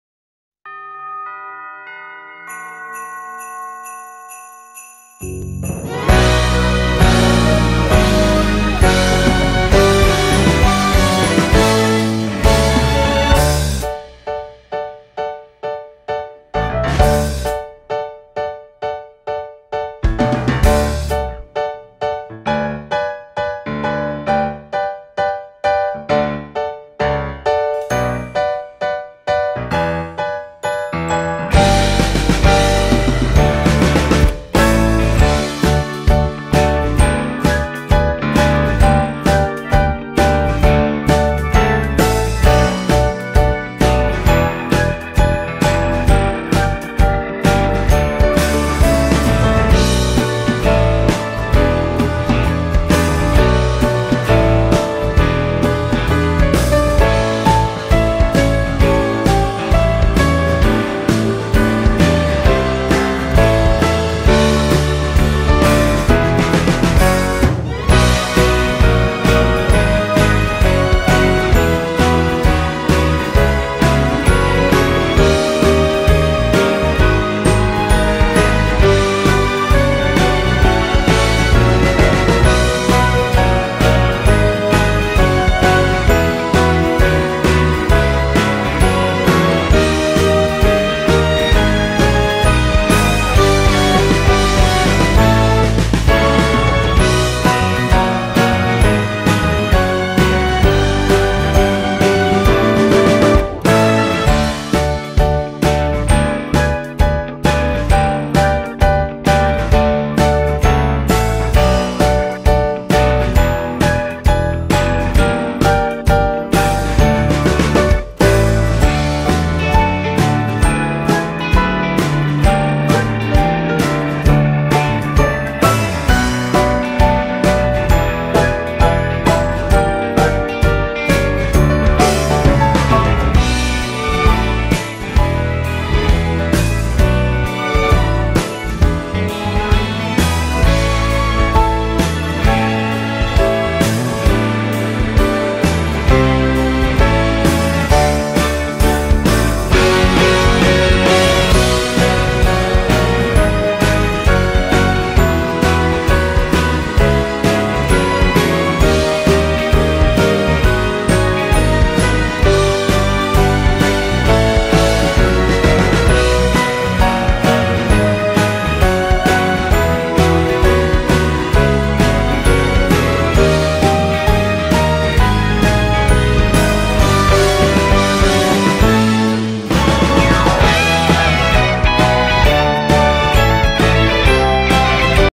Ensemble Stars - Tomoya Mashiro - Christmas Speedpaint
Merry Christmas and Happy Holidays everyone! This year,  I drew Tomoya Mashiro for Christmas :)